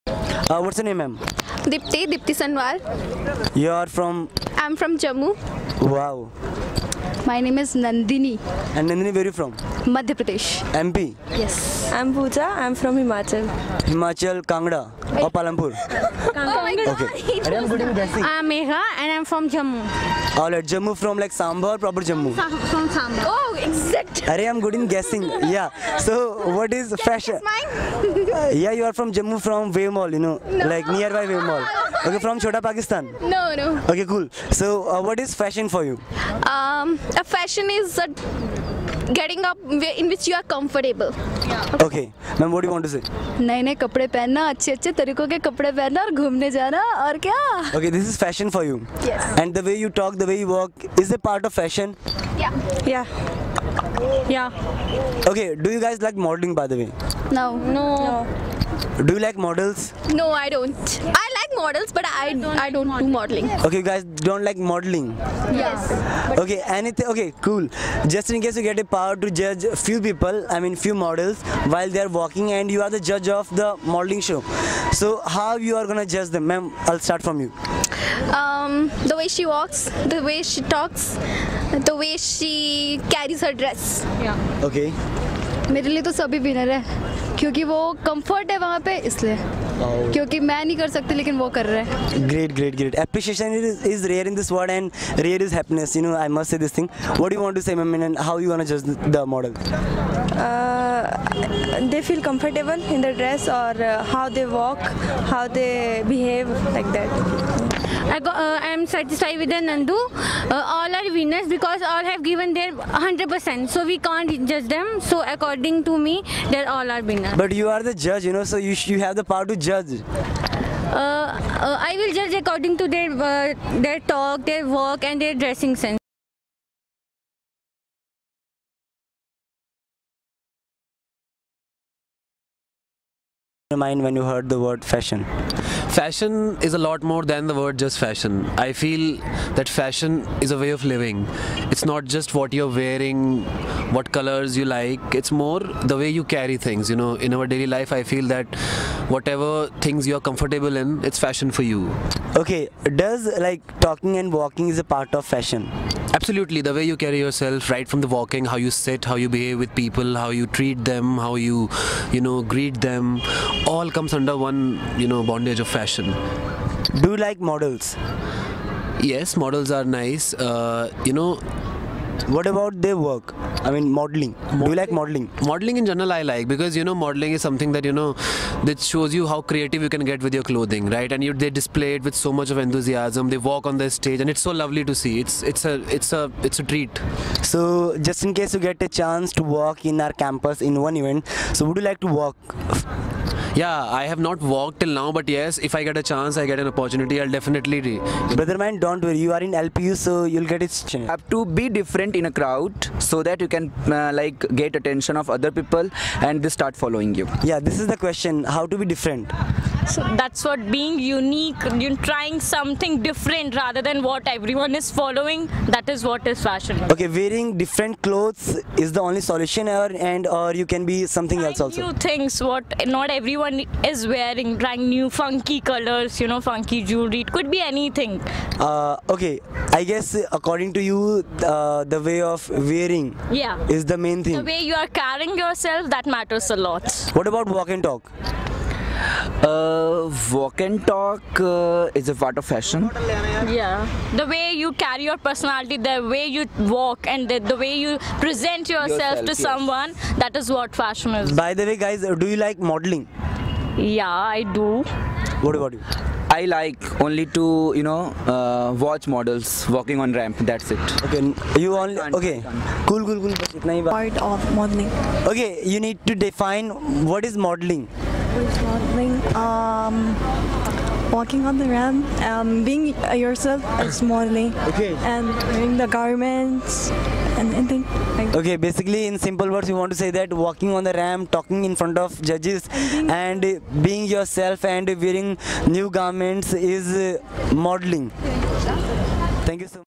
BOYS & GIRLS ON FASHION || SHIVAM RANA || K.D.S
HEY BEAUTIFUL PEOPLE OUT THERE ...
CHECK WHAT LPU GUYS KNOW ABOUT FASHION .